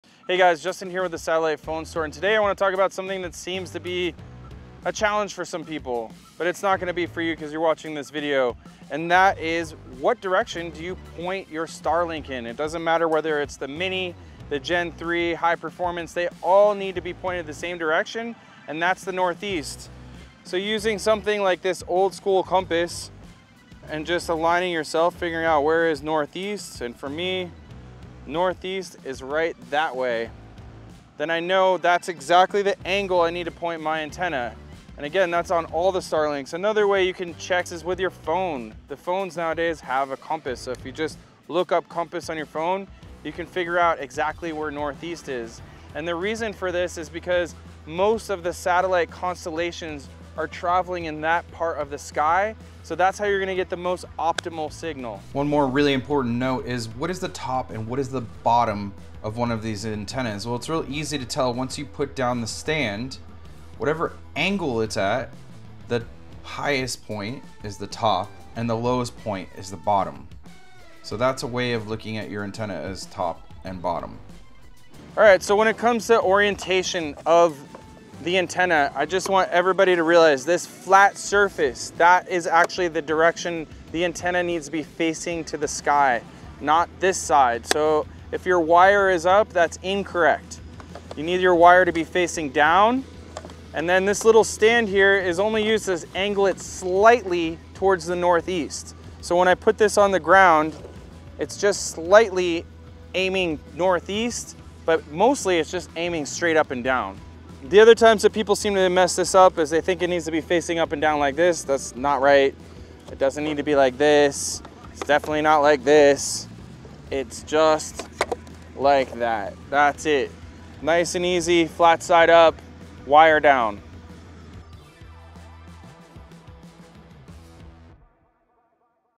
Proper Starlink Alignment! - how to align your starlink antenna
Justin from Satellite Phone Store explains how to point your Starlink dish for optimal signal. Whether it's the Mini, Gen 3, or High Performance, they all need to point Northeast. Use a compass or your phone's compass app for easy alignment to the northeast to get the most out of your Starlink internet and Starlink satellite.